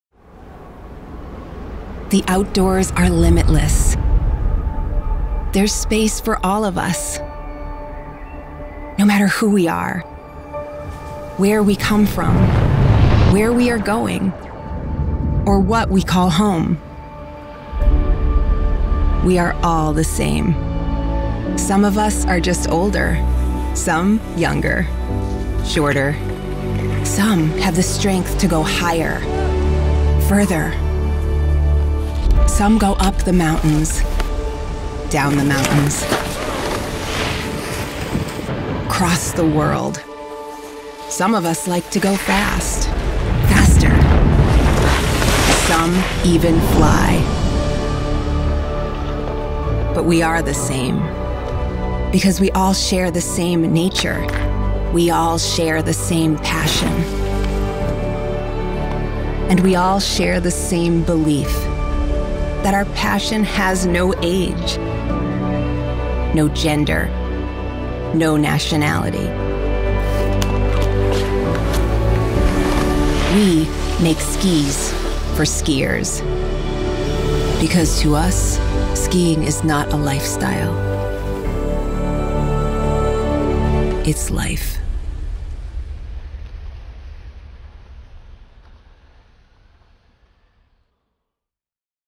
Fischer Skis
Inspirational Video about the LOVE of Skiing. This is a Video I voiced for Fischer Skis. To learn more about me or to collaborate, please visit my